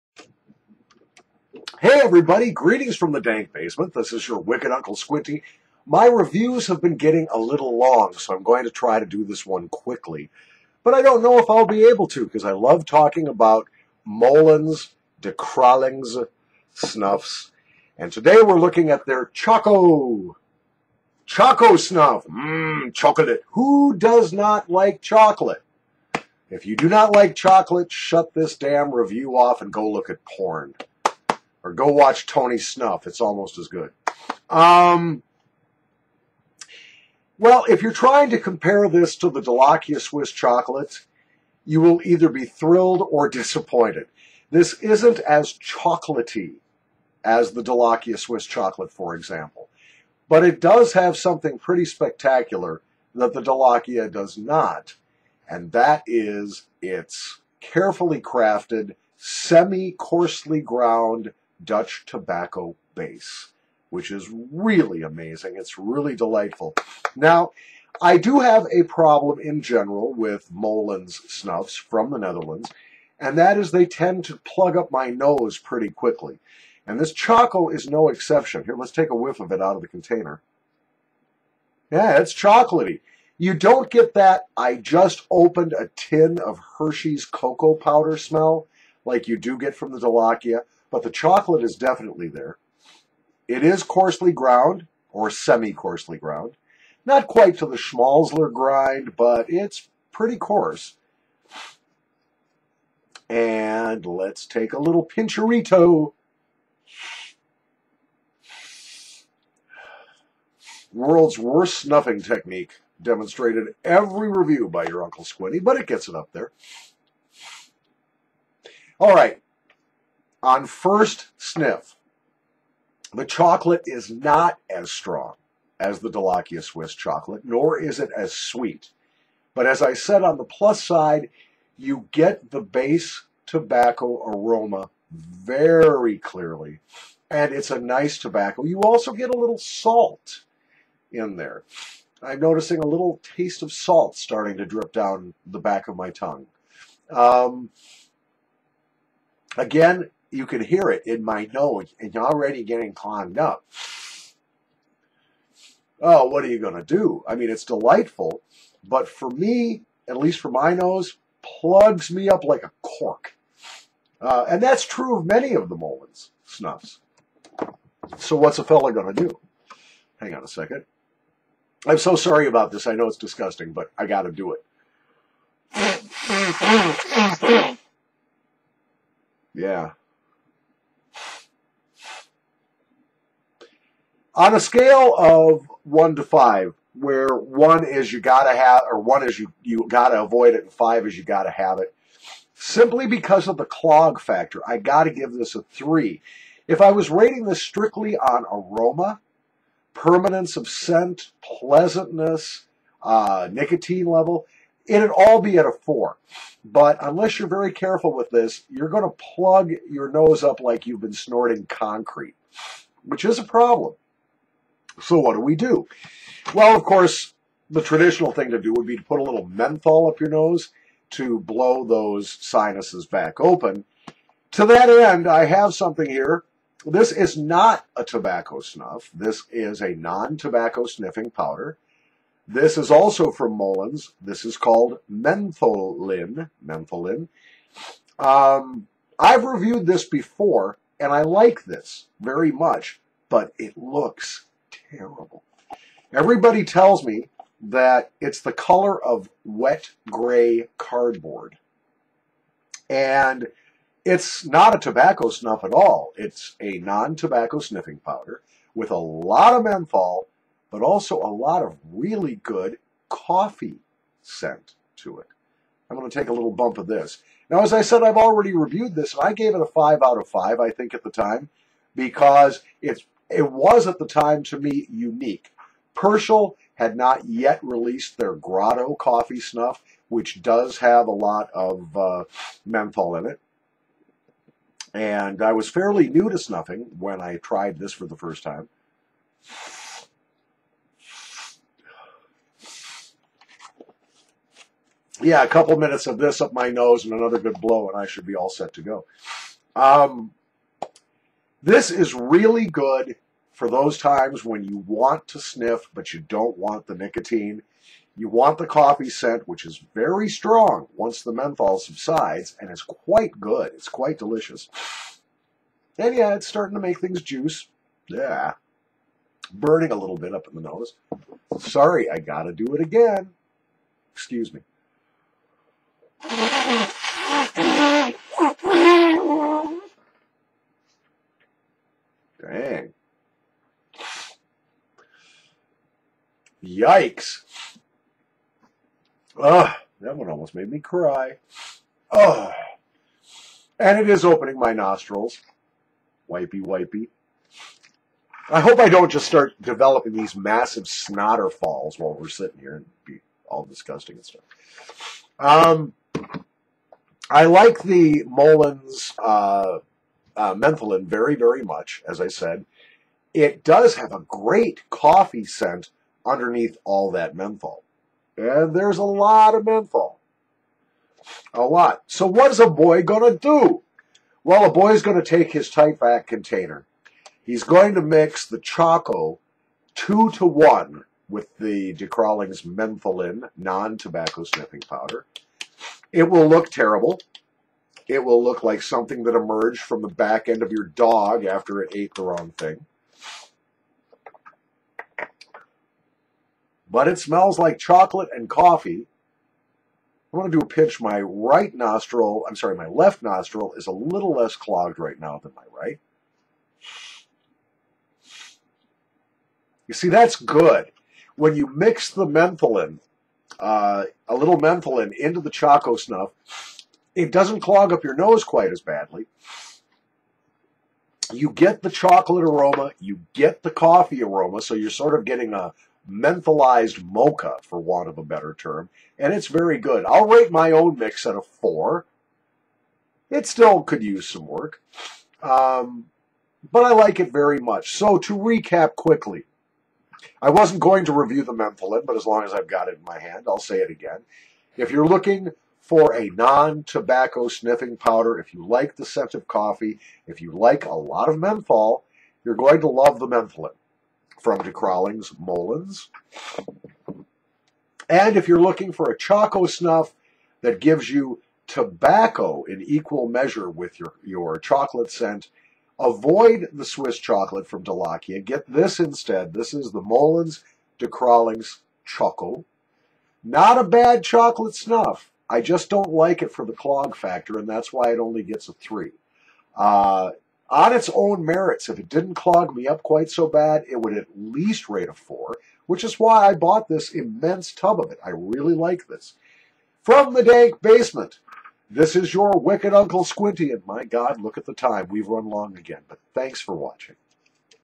Oct 15 13 Molens Choco Nasal Snuff Review Plus
I review the Choco snuff from Molens, plus comment on their non-tobacco sniff "Mentholin" and then take a pinch from a mix of the two.